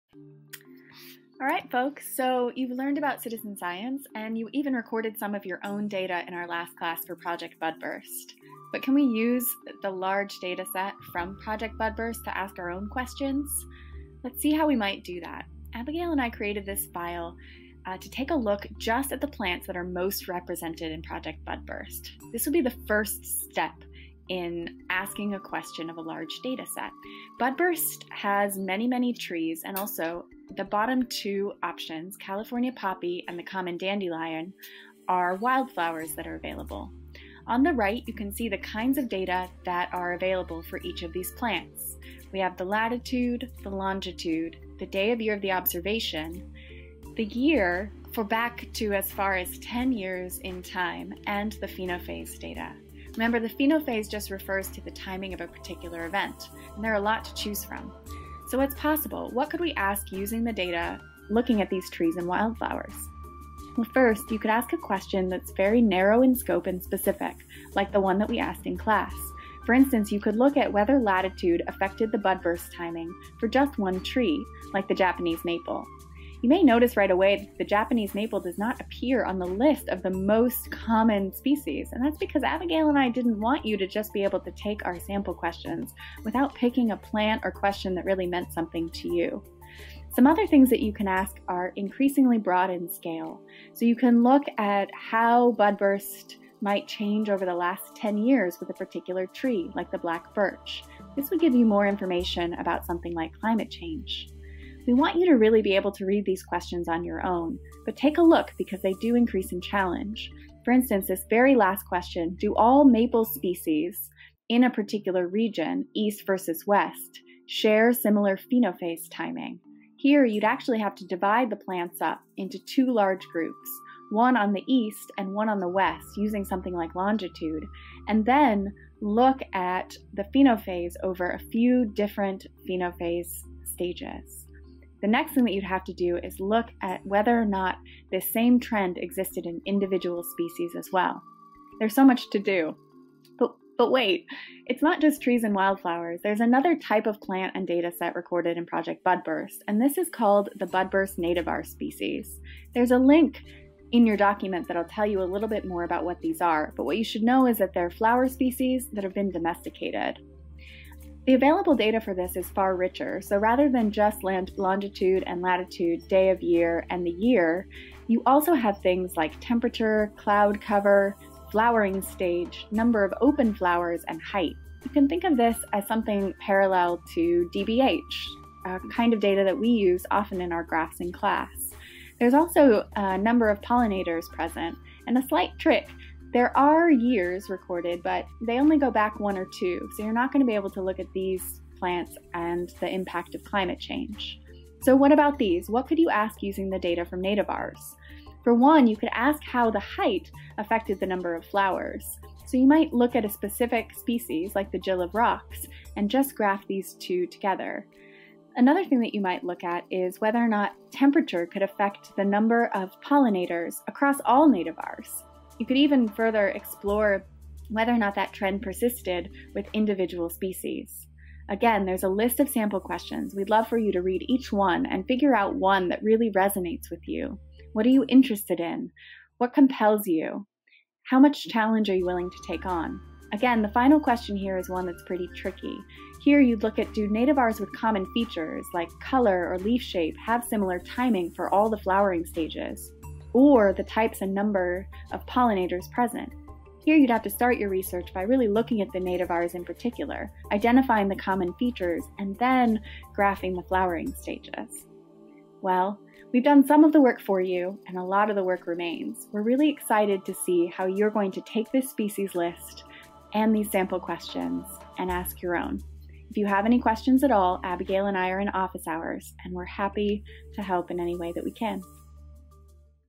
Species and Sample Questions
Asking questions using the Budburst data set